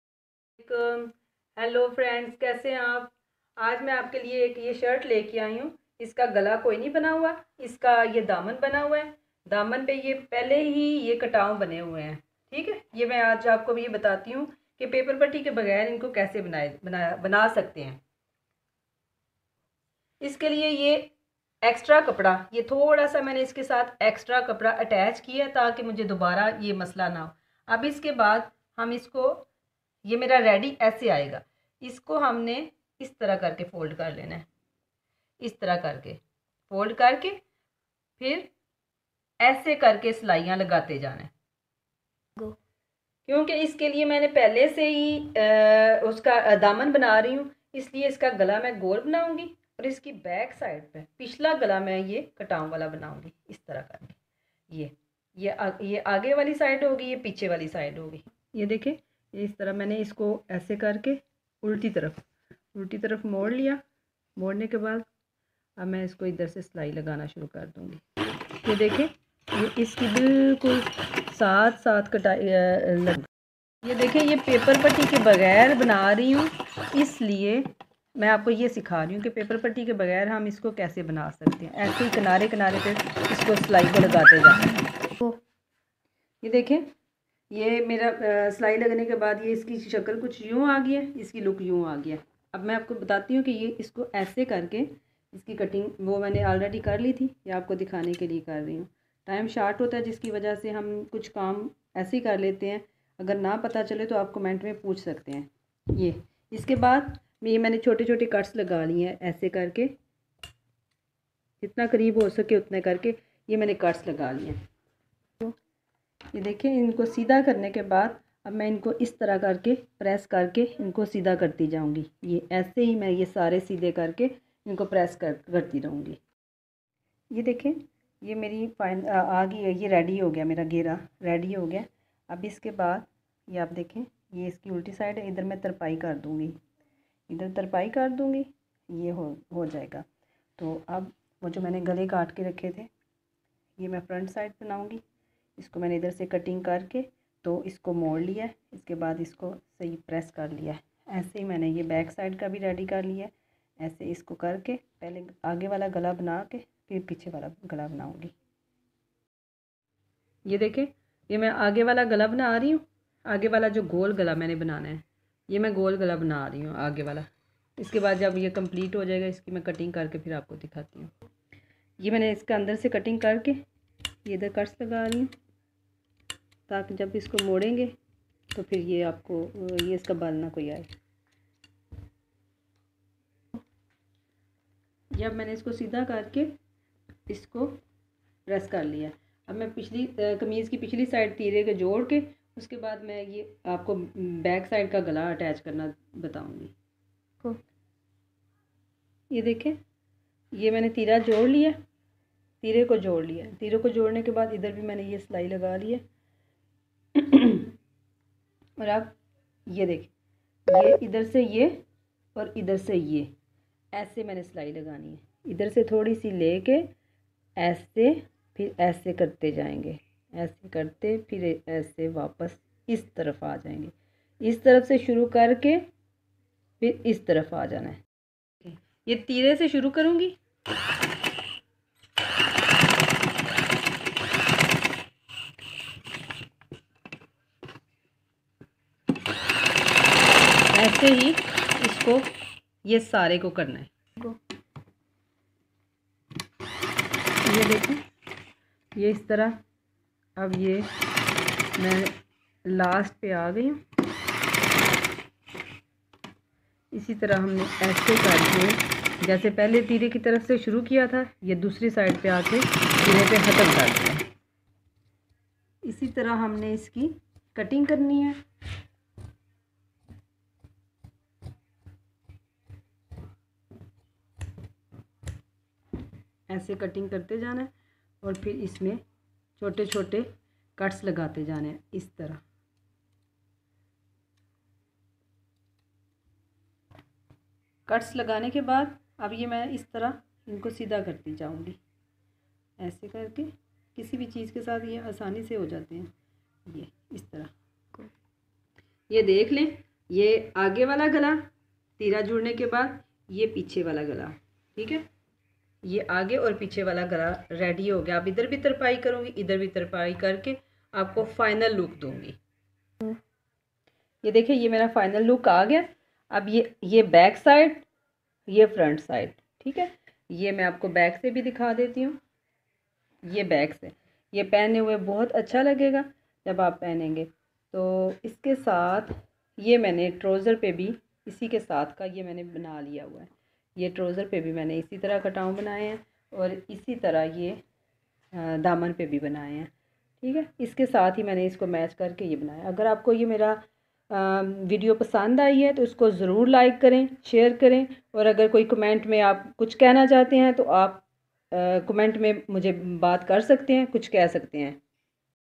paperpattikaybagair daman bnany Ka treeky mukamal /pichla khubsurat stylish ghala nanny Ka treeka
paperpatti Kay bagair dam bnany Ka mukamal treeka aur khubsurat pichla ghala bnany Ka mukamal treeka